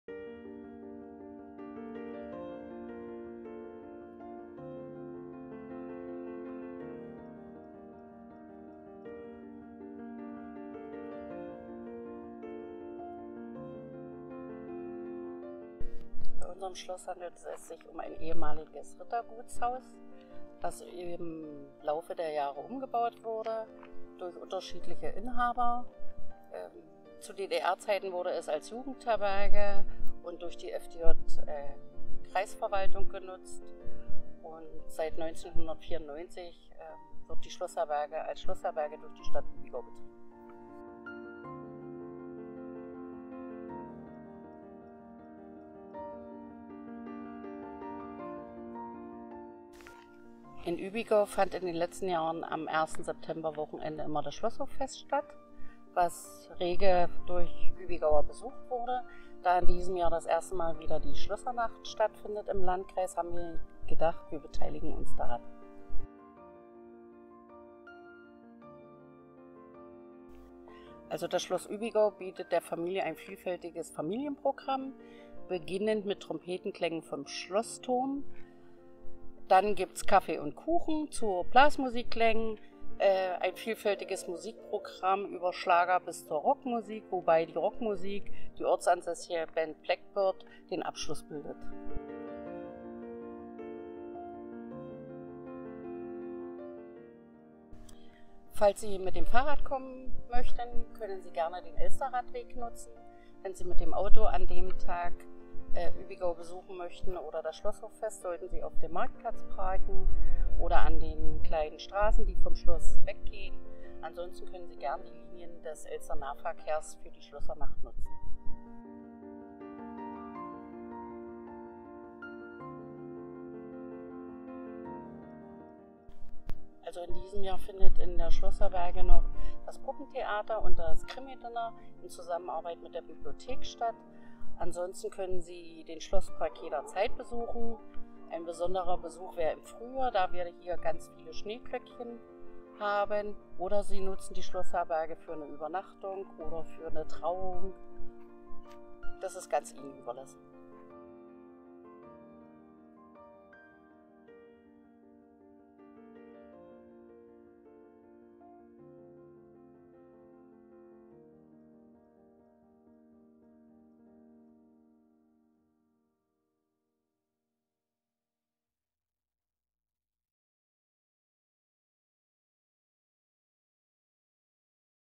Schlössernacht - Schloss Uebigau
Die Tore öffnen sich - eine Veranstaltung die den Landkreis Elbe-Elster vereint

Die Elbe-Elster Schlössernacht verwandelt die Region am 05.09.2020 in eine riesige Open-Air Kultur-Landschaft!

Was sie im Einzelnen für kulturelle Überraschungen bereithält, erfahren Sie auf der gleichnamigen Facebook-Seite Elbe-Elster Schlössernacht oder

Um Ihnen einen Überblick zu verschaffen, ist unser Social-Media Team zu den Schlössern gereist und hat dort zusammen mit dem Tourismusverband Interviews geführt.

Unsere Reise zur Vorbereitung der Schlössernacht führte uns auch nach Uebigau. Die dort liegende Schlossherberge liegt in der Nähe des Marktplatzes.

Zur Schlössernacht erwarten Sie Trompetenklänge
vom Schlossturm, musikalische Klänge von den
„Namenlosen‘“ sowie am Abend das Konzert der einheimischen Band „Black Bird“ im Schlosshof. Außerdem erwarten Sie
Turmführungen, eine Hüpfburg für Kinder, Kinderschminken, eine Kaffeetafel,
Deftiges vom Grill und Getränke.

Video: LKEE Social-Media Team
Grafik Schlössernacht: studio for interactive media Christian Becker
Elbe-Elster / Elbe-Elster Tourismus Logo: diepiktografen.de